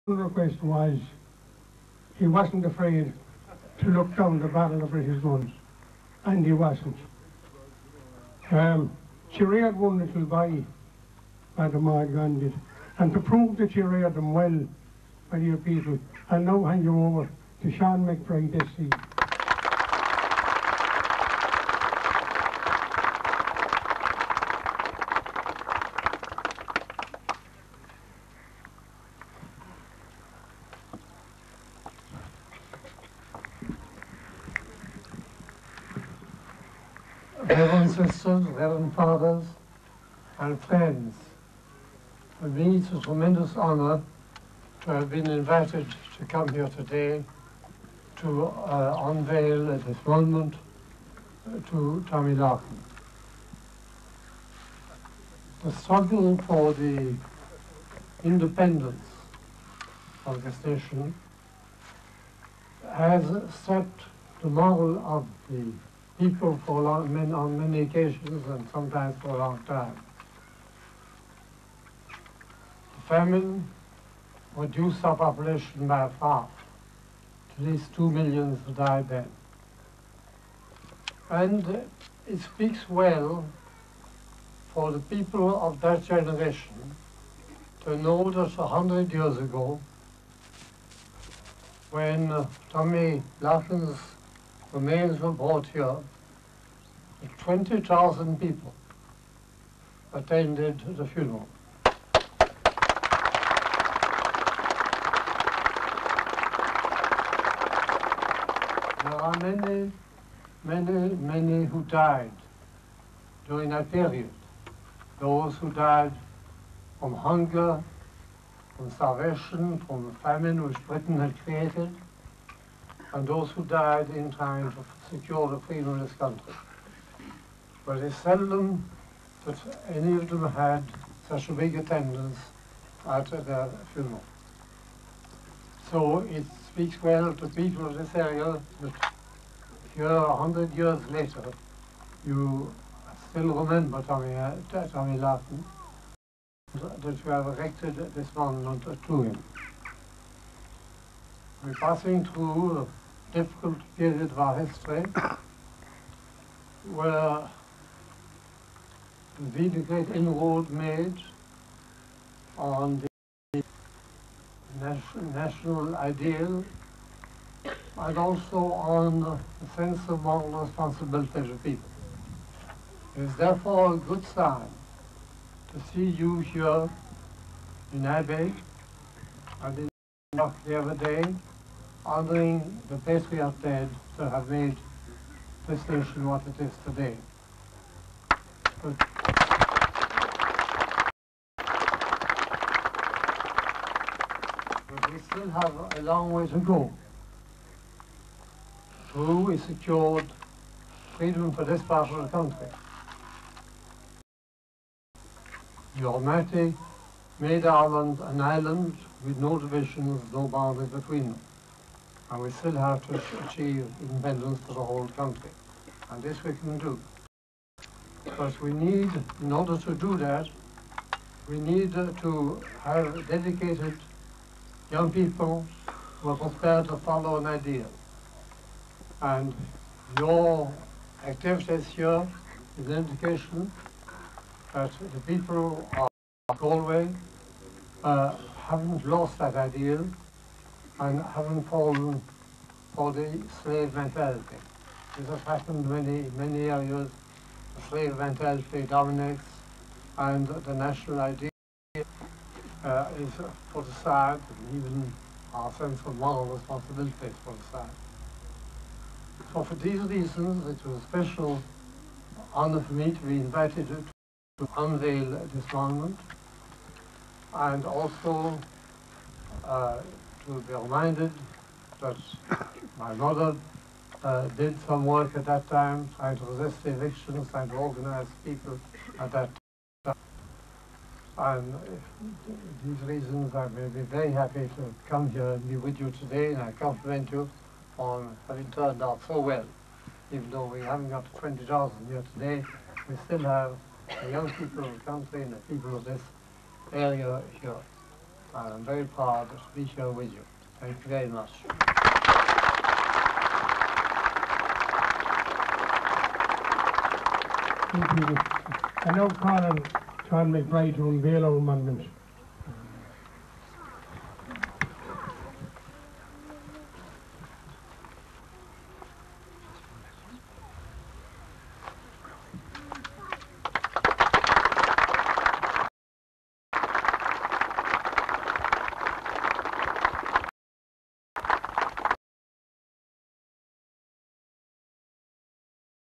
Sean MacBride Co Galway 1986 Tommy Larkin Comemoration Speech, Abbey, Woodford, Co Galway
Sean Mac Bride speaks at commemoration for Tommy Larkin at Abbey, Woodford, Co Galway, August 1986. (Sean MacBride awarded Nobel Peace Prize 1974. Key figure Amnesty International 1970's). Tommy Larkin intervened on a childs behalf during a tenant eviction 130 years ago after she had been roughly treated by a Policeman, and was jailed in Kilkenny where he died from neglect. His heroic action resulted in 5,000 turning out for his funeral cortege in Kilkenny, 5,000 in Birr, and 10,000 in Woodford, Co Galway. This united the tenants against their landlords.
"Battle of Saunders' Fort" - the eviction of Thomas Saunders, one of Lord Clanricarde's tenants, Woodford, Galway, August 1886.  Interviewer Micheal O'Conaill. Video August 1986 filmed by Richard Murphy, Athenry.